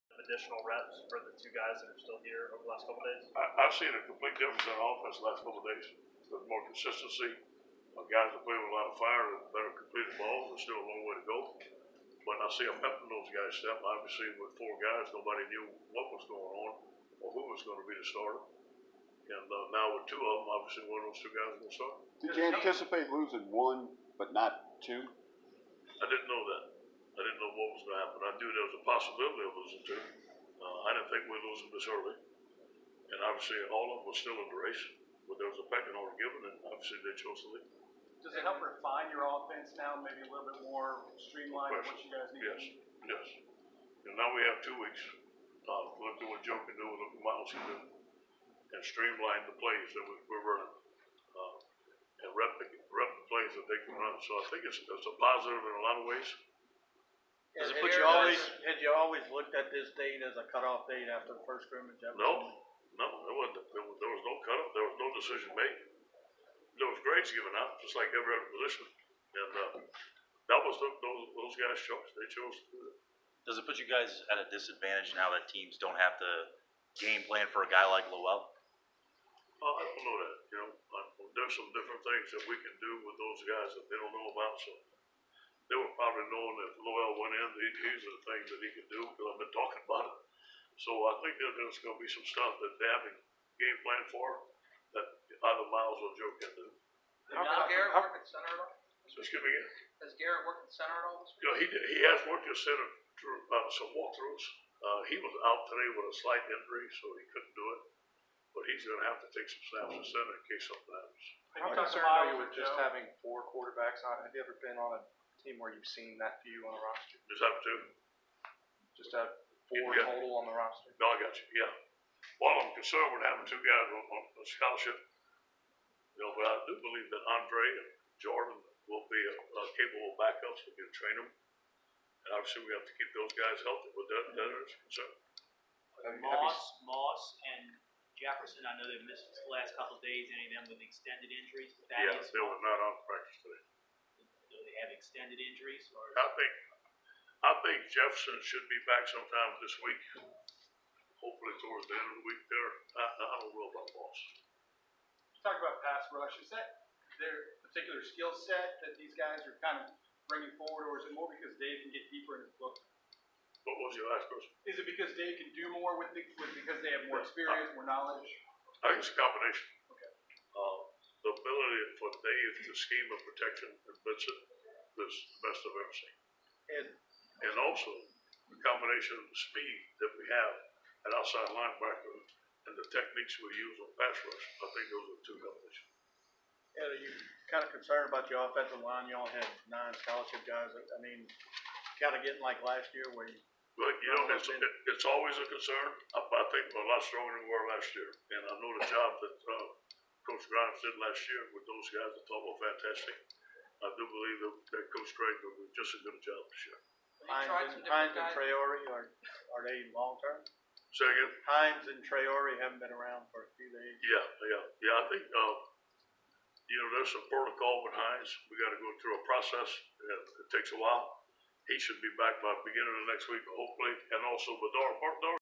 Ed Orgeron goes more in depth on LSU QB battle following transfers
The departures of Justin McMillan, Lowell Narcisse leave Joe Burrow, Myles Brennan to man the Tigers' quarterback duties